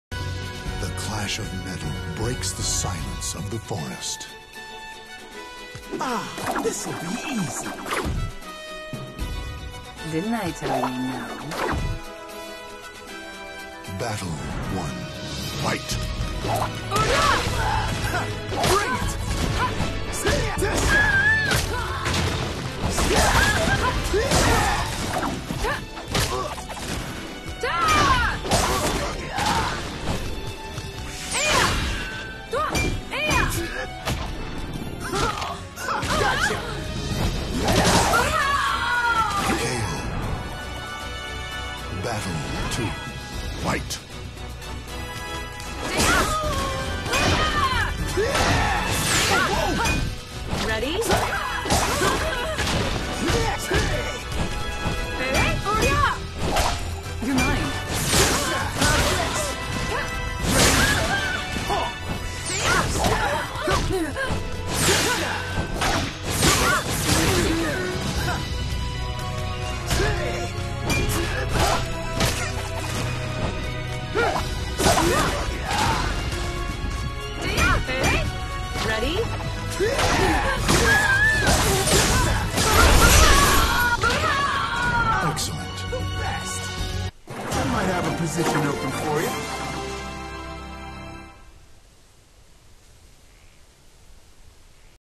Strikers vs. DXG (Battle #22): Ryo vs. Li
It's the 2nd meeting between Ryo and Li, and they haven't face each other since the 2nd Soul Fest. In that encounter, the battle was very tight. However, Li was able to hold Ryo long enough to win the battle. Can Ryo bounce back or Li gain another victory?